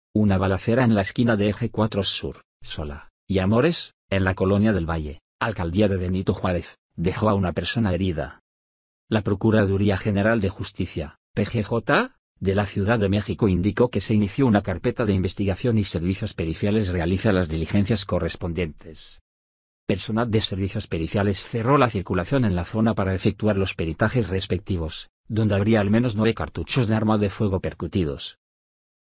Balacera en la Del Valle deja una persona herida
Una balacera en la esquina de Eje 4 Sur, Xola, y Amores, en la colonia Del Valle, alcaldía de Benito Juárez, dejó a una persona herida.

La Procuraduría General de Justicia (PGJ) de la Ciudad de México indicó que se inició una carpeta de investigación y Servicios Periciales realiza las diligencias correspondientes.

Personal de Servicios Periciales cerró la circulación en la zona para efectuar los peritajes respectivos, donde habría al menos nueve cartuchos de arma de fuego percutidos.